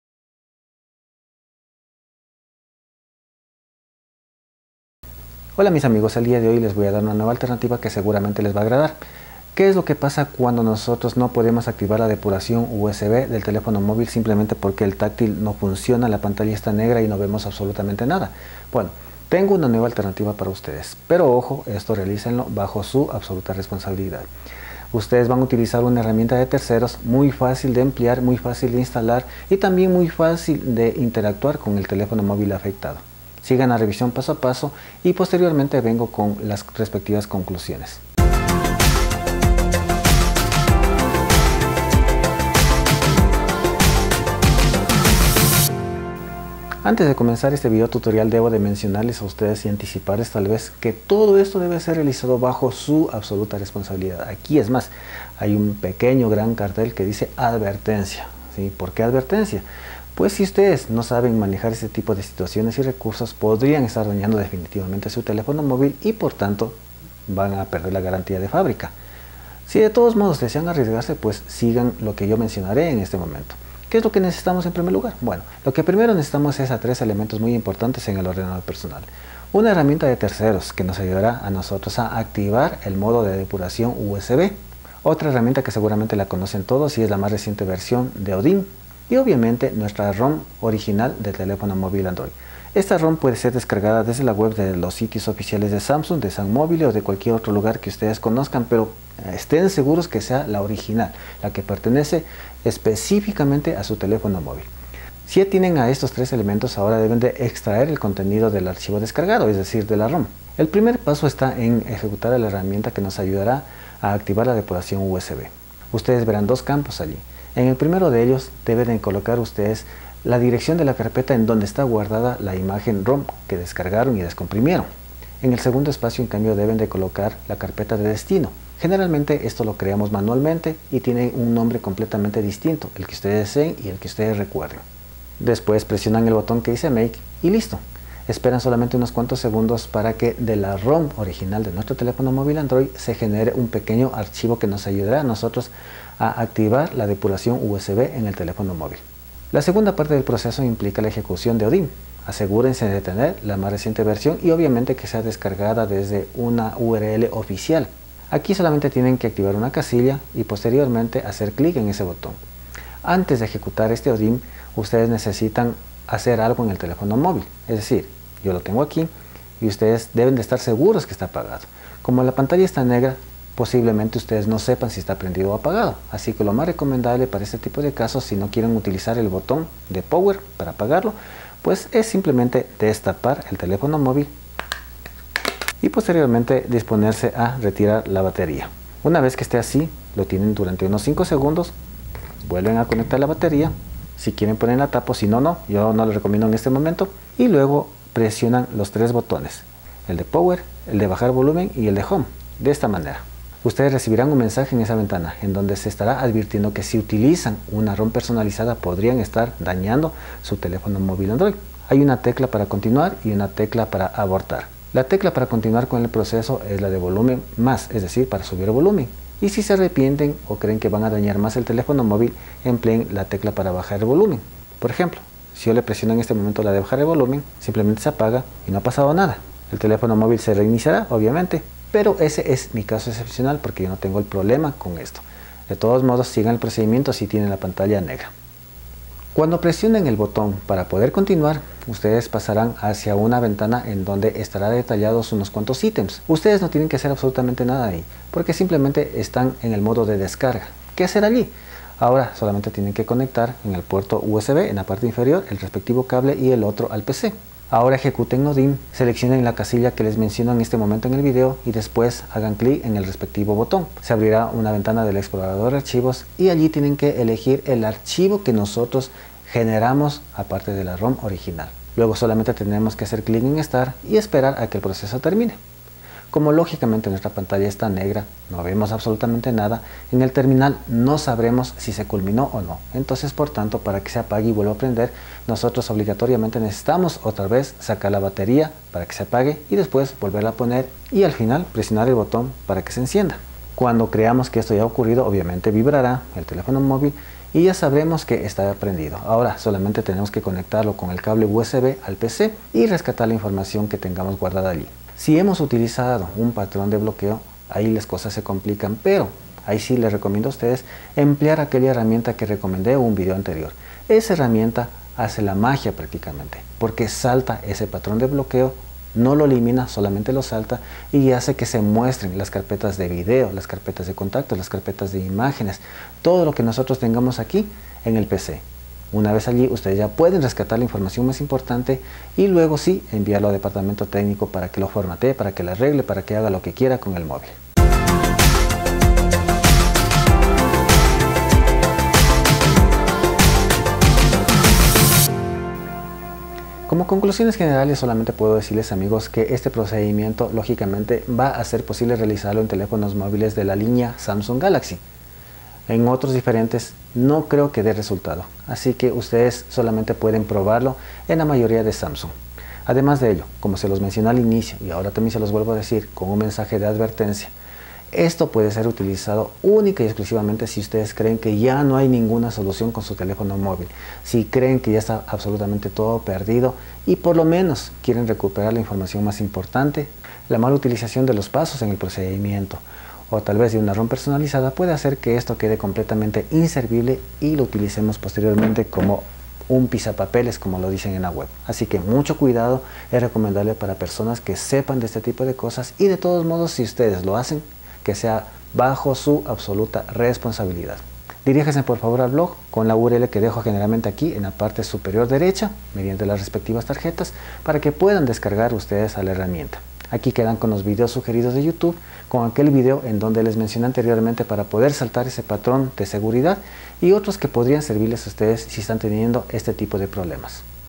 Activar Depuracion USB Android con Pantalla Negra | Somos Android
► Descripción:
En Somos Android damos una nueva alternativa para quienes tienen un teléfono móvil Android con la pantalla negra, pantalla rota o tácil dañado, para que intenten recuperar las fotos y videos de la SD interna.

El método está recomendado para algunos modelos de teléfonos móvil de la línea Samsung Galxy.

El objetivo principal será, intentar activar la depuración USB en Android, cuando el teléfono móvil tienen la pantalla negra.

ADVERTENCIA:

Usa éste método y sus pasos (del video) bajo tu responsabilidad. No nos responsabilizamos por cualquier efecto dañino que pueda inhabilitar por completo del terminal.

ALL Android.

ALL Windows...

CHATEANDO